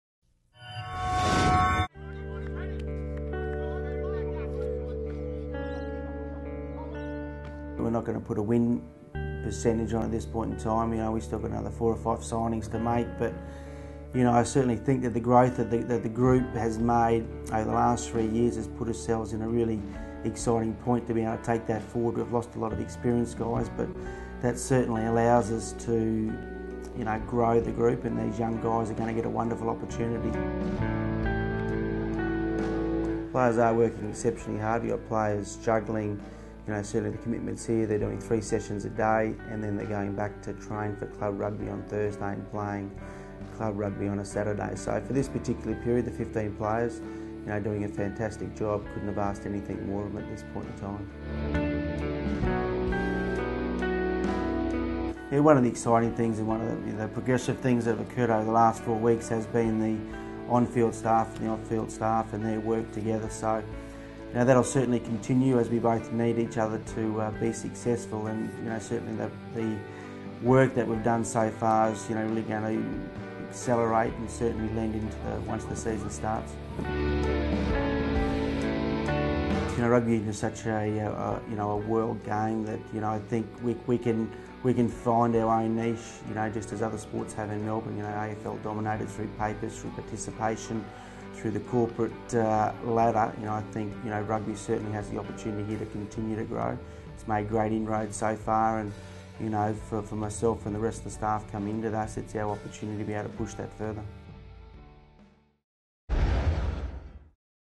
REB TV meets Tony McGahan part two
We sat down with Tony to talk about how the squad is going, and where he sees the Rebels heading in 2014.